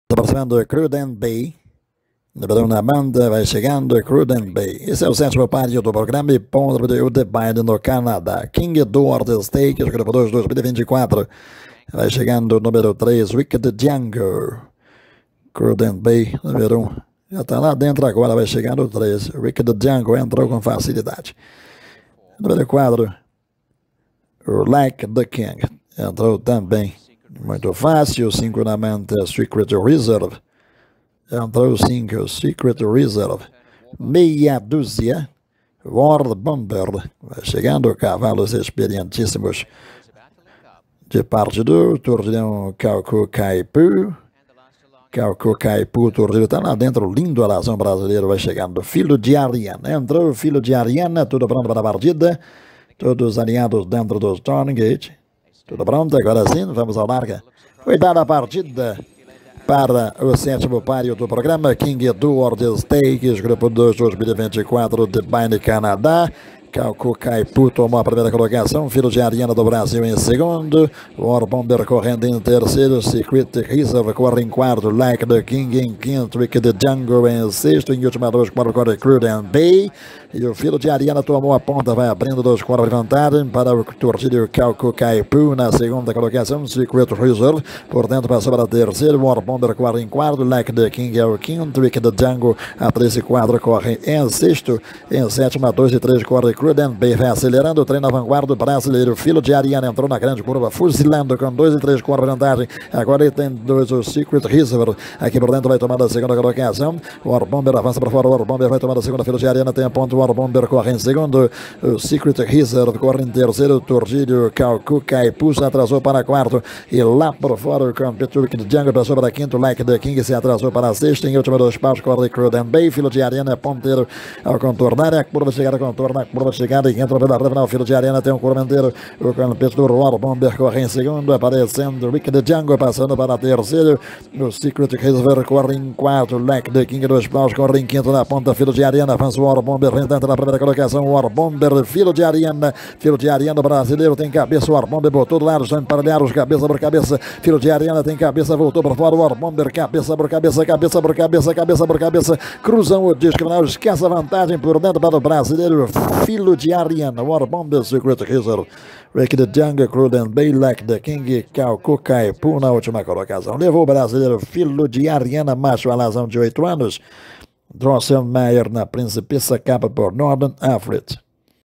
King Edward Stakes (G2) 2024 - Filo Di Arianna (BRZ)
Track: Woodbine (CAN)
Winner: Filo Di Arianna (Drosselmeyer - Pricipessa Capri  by  Norhern Afleet)
Breeder: Stud Principessa Di Capri
Owner: Gary Barber - Wachtel Stables - Peter Deutsch
Trainer: Mark Casse
Jockey: Sahin Civaci
Voice Over: Marcelo Freitas (Portuguese Version)